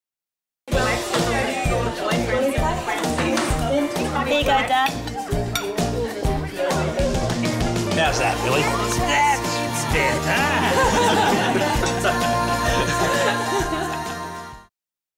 For Cooking Appliances and more... That's Betta
Betta Home Living - Brand Ad for Cooking

Visit us online or in store to browse our great range of cooking appliances. Follow the below link to our great range of ovens;